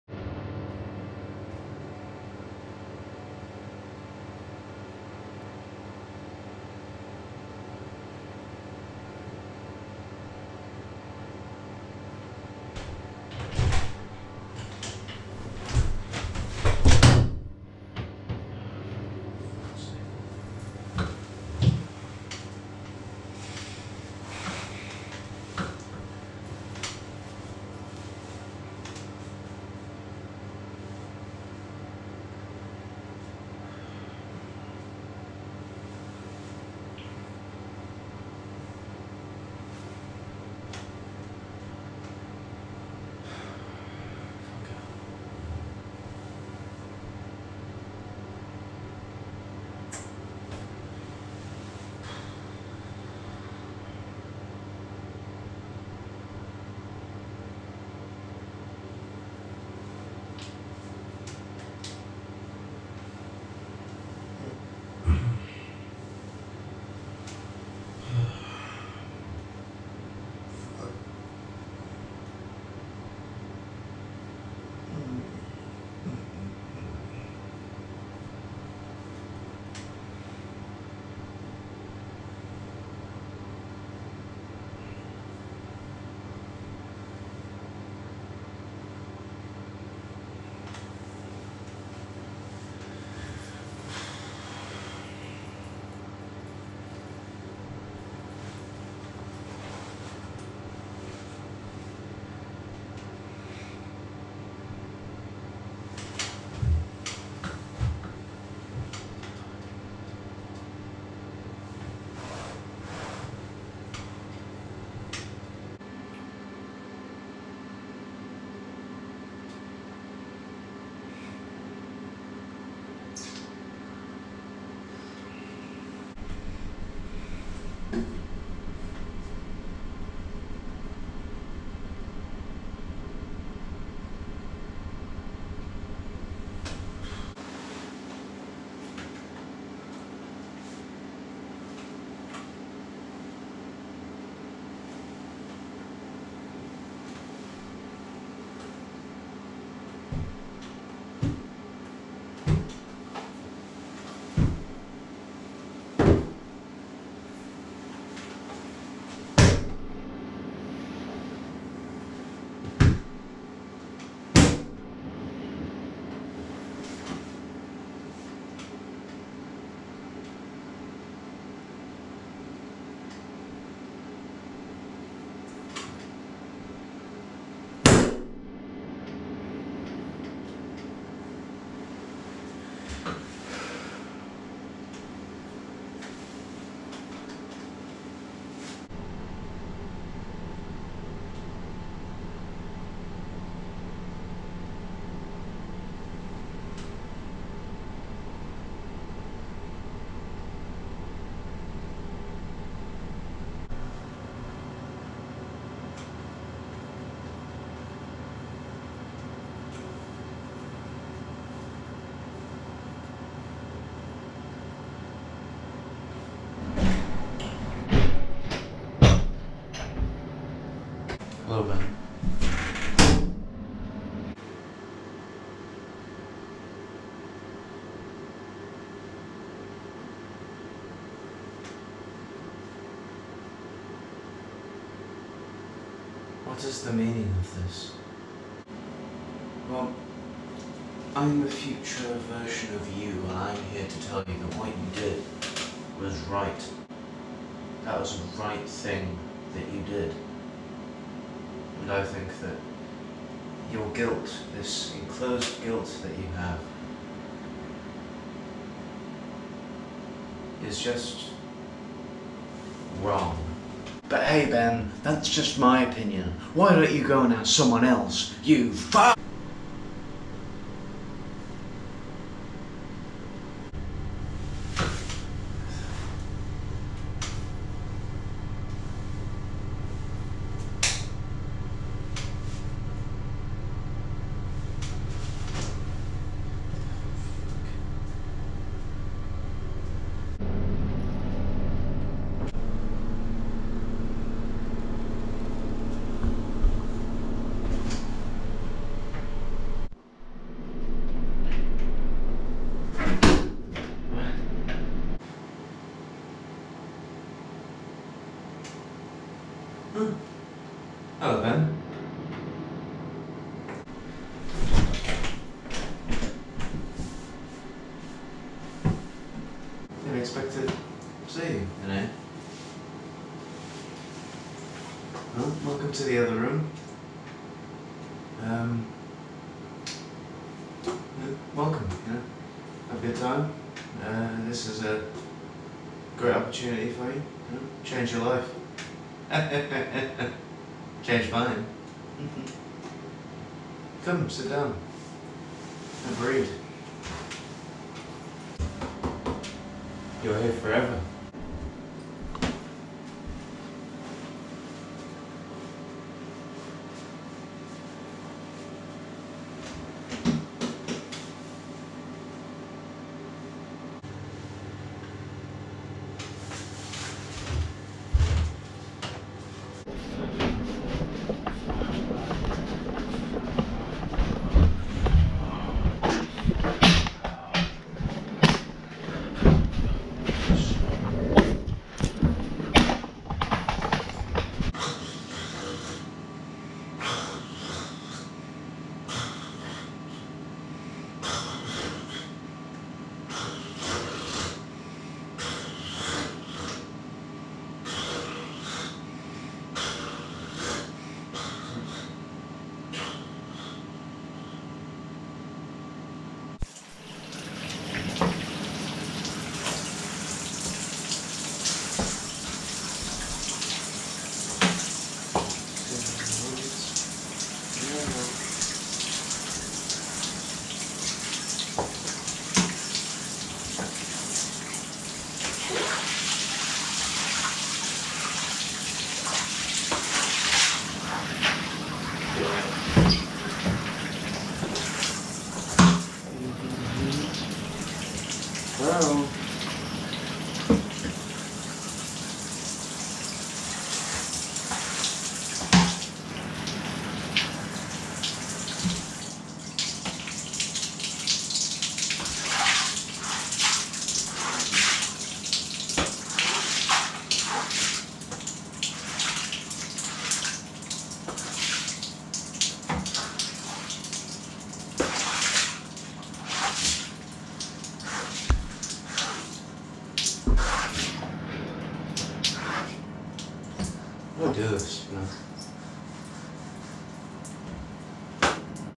The Other Room (2022)
The mind is like 2 rooms, one quiet and reserved, the other wild and savage, which side are you on?